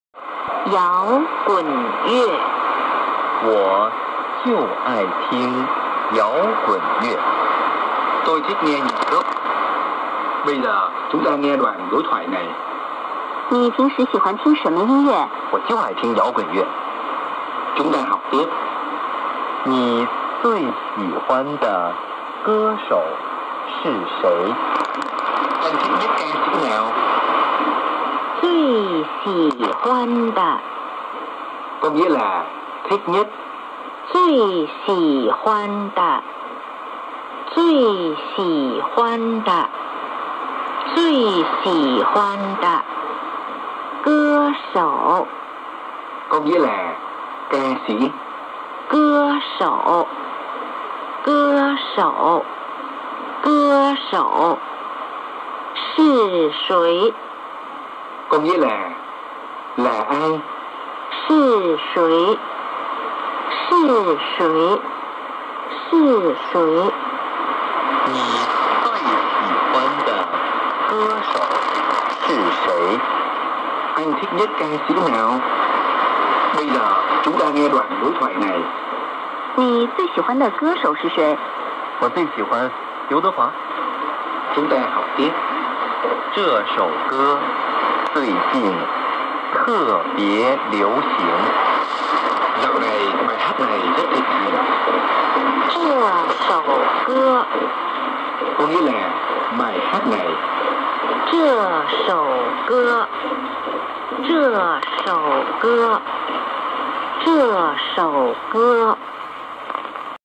17/12/2021 [MW DX] CRI MW 1296 kHz Vietnamese Received in Langkawi, Kedah 🇲🇾
• Radio Station : China Radio International
• Power : 600 kW
• Frequency : MW 1296 kHz
• Transmission Location : NRTA Transmitting Station 501, Pingdingshan, Siyi Village, Anning City, Kunming, Yunnan Province 🇨🇳
• QTH : Langkawi Island, Kedah State 🇲🇾
• Distance : 2,067 KM
• Language : Vietnamese
• Time : 16:50 UTC / 00:50 UTC +8 Malaysia Time
• Date : 17 December 2021
• Receiver : XHDATA D-808 (DSP)
• Propagation : Skywaves